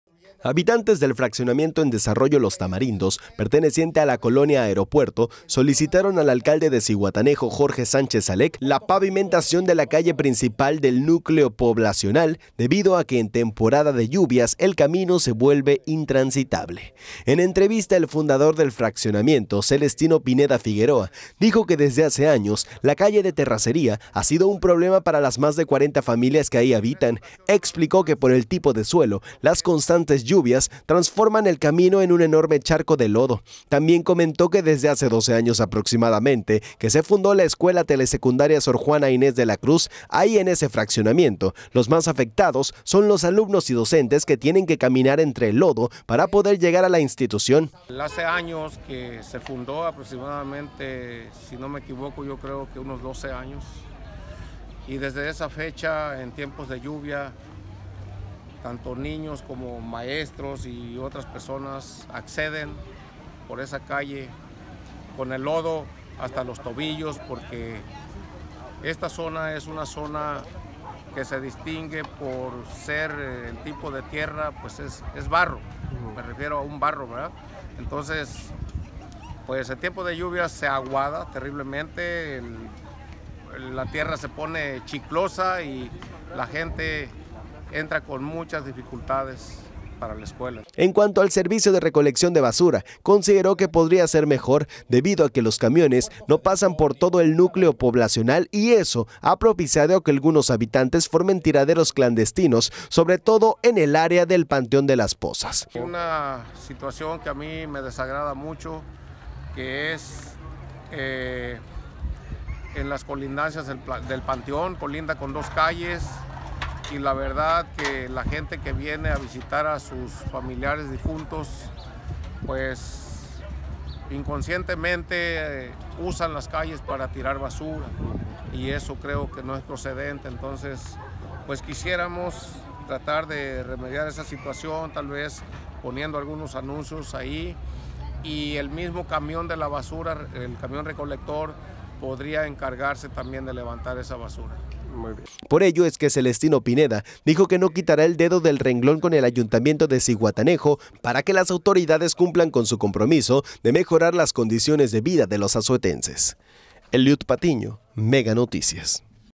Piden pavimentación de calle principal en Los Tamarindos
Habitantes del fraccionamiento en desarrollo Los Tamarindos, perteneciente a la colonia Aeropuerto solicitaron al alcalde de Zihuatanejo, Jorge Sánchez Allec la pavimentación de la calle principal del núcleo poblacional, debido a que en temporada de lluvias el camino se vuelve intransitable.